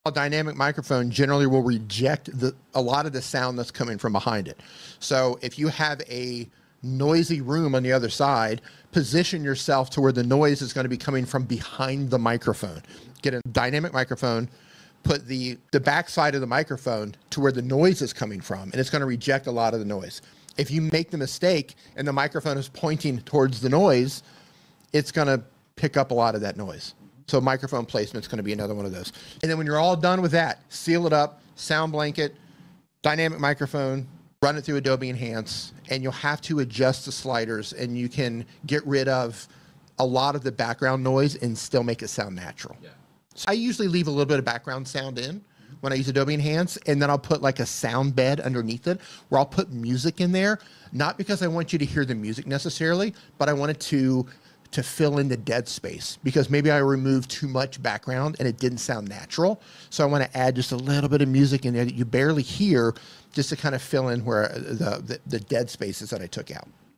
Eliminate Background Noise with Dynamic Microphones
Using the right Microphone for the right situation can make or break your audio.

Do you want your YouTube question answered? I go live on my channel @NickNimmin to do just that. Every Saturday 9am Eastern.